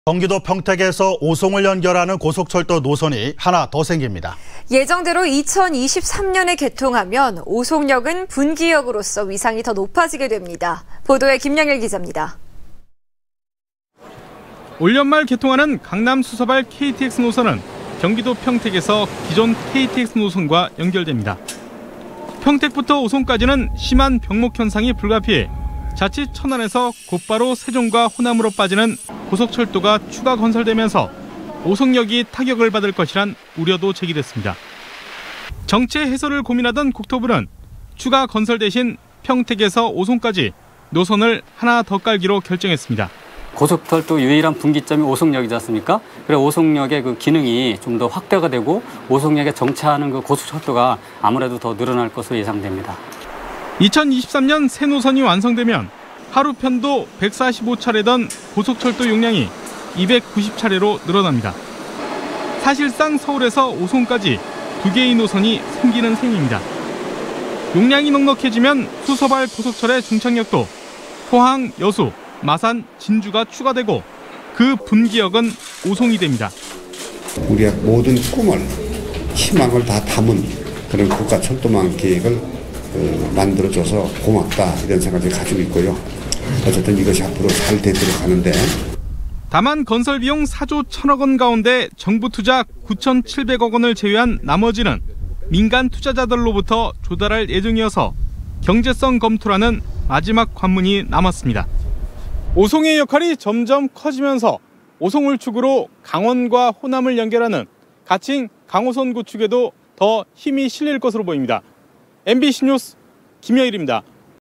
[청주MBC뉴스] KTX 평택-오송 복복선 추진
◀앵커▶
 경기도 평택에서 오송을 연결하는
고속철도 노선이 하나 더 생깁니다.
 예정대로 2023년 개통하면 오송역은
분기역으로 위상이 더 높이지게 됩니다.
 김영일 기잡니다.

           ◀ＶＣＲ▶
 올 연말 개통하는 강남 수서발 KTX 노선은
경기도 평택에서 기존 KTX 노선과 연결됩니다.

 평택부터 오송까지는 심한 병목 현상이
불가피해 자칫 천안에서 곧바로 세종과
호남으로 빠지는 고속철도가 추가 건설되면서
오송역이 타격을 받을 것이란 우려도
제기됐습니다.

 정체 해소를 고민하던 국토부는
추가 건설 대신 평택에서 오송까지
노선을 하나 더 깔기로 결정했습니다.

           ◀ＩＮＴ▶
  고근석 충청북도 교통물류과장

 2023년 새 노선이 완성되면 하루 편도
145회던 고속철도 용량이 290회로 늘어납니다.

 사실상 서울에서 오송까지 2개의 노선이
생기는 셈입니다.

 용량이 넉넉해지면 수서발 고속철의 종착역도
포항,여수,마산,진주가 추가되고
그 분기역은 오송이 됩니다.

            ◀ＩＮＴ▶
           이시종 도지사

 다만, 건설비용 4조 천 원 가운데
정부 투자 9천7백억 원을 제외한 나머지는
민간 투자자들로부터 조달할 예정이어서
경제성 검토라는 마지막 관문이 남았습니다.

S/U"오송의 역할이 점점 커지면서 오송을
축으로 강원과 호남을 연결하는 가칭 강호선
구축에도 더 힘이 실릴 것으로 보입니다.
MBC NEWS 김영일입니다."
           ◀ＥＮＤ▶